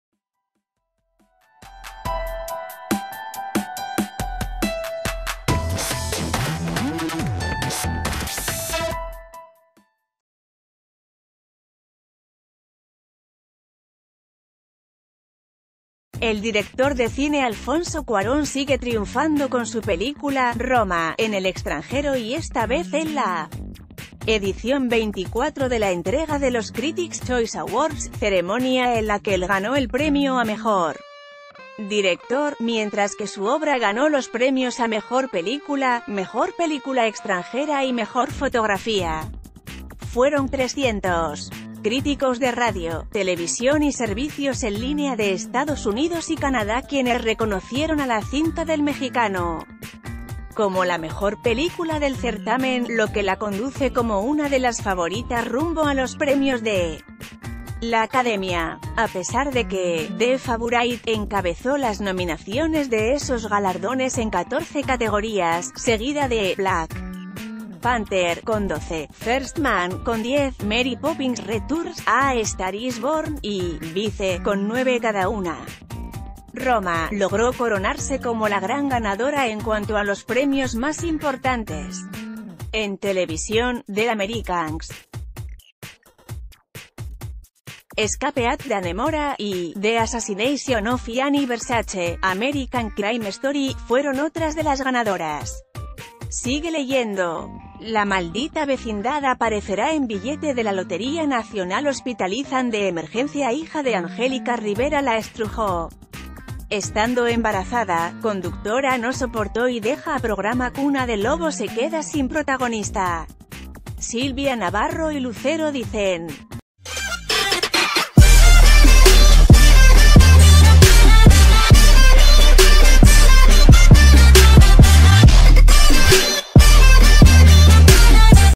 Alfonso Cuarón, mejor director en Critics’ Choice Awards por Roma
El director de cine Alfonso Cuarón sigue triunfando con su película “Roma” en el extranjero y esta vez en la edición 24 de la entrega de los Critics’ Choice Awards, ceremonia en la que él ganó el premio a Mejor Director; mientras que su obra ganó los premios a Mejor Película, Mejor Película Extranje...